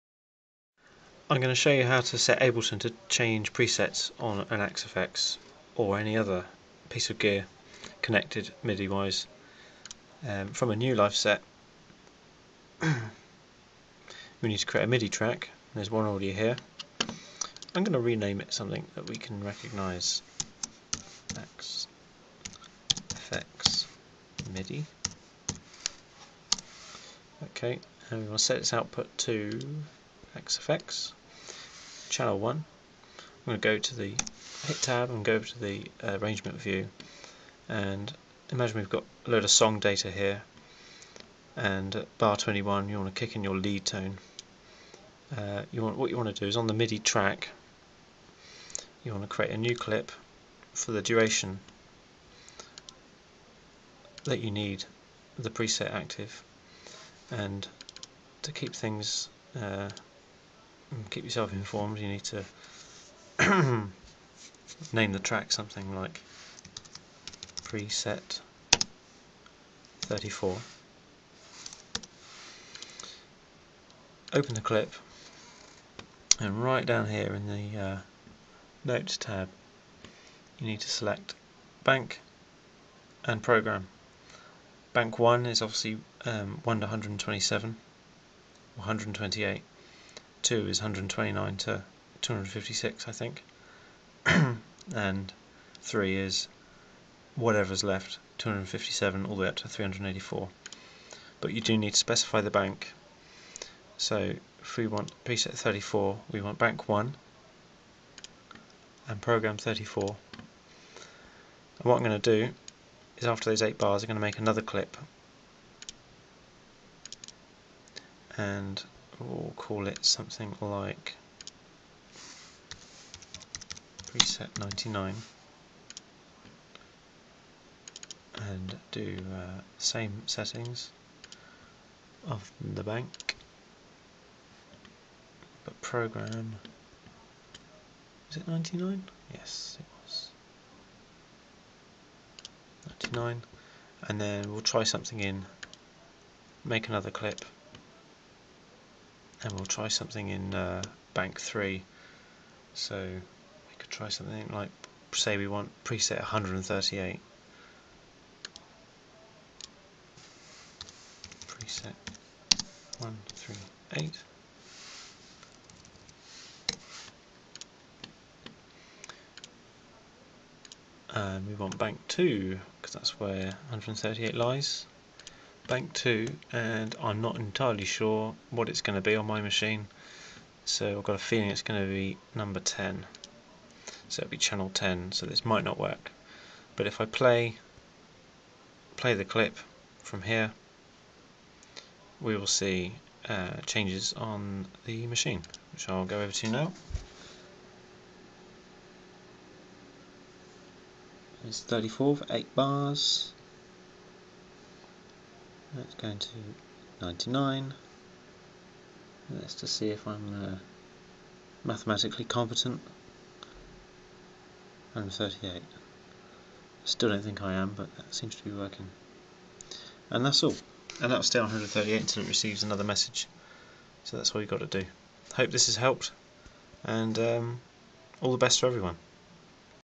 Automating axe fx preset in Ableton Live
A tutorial explain how to set ableton live to change preset on your axe fx via a midi track.